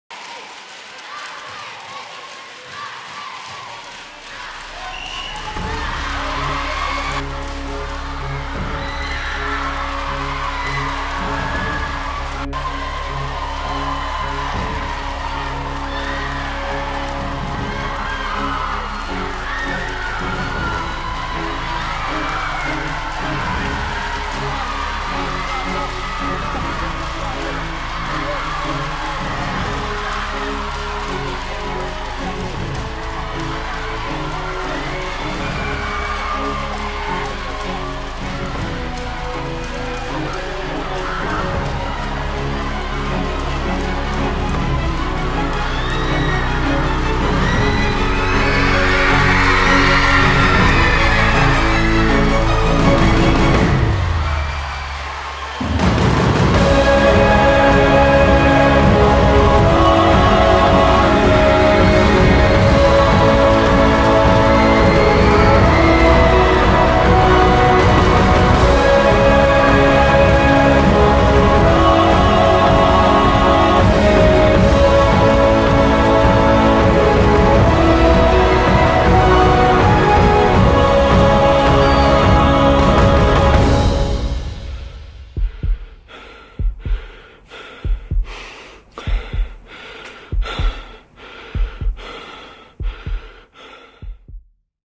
Айбатыр МЫРЗАБЕКОВ жеңіс үшін күрескен ержүрек бала (04.11.16)
Біз бұл керемет видеоға мән бермей қоя алмадық! Батыс Қазақстан облысында туған бала спорттық ойынға қатысу арқылы жұртты тамсандырды. Бұл видеодағы драма мен құмарлық гладиаторлар туралы кең форматты боевик түсіруге жетер еді. Айбатыр МЫРЗАБЕКОВ жеңіс үшін күресу керектігін көрсетті. Осы видеоны көруге кеңес береміз. Қуанышпен қатар көзге жас алатындарыңызға сенімдіміз. Бұл біздің адам!Бұл қазақтың баласы!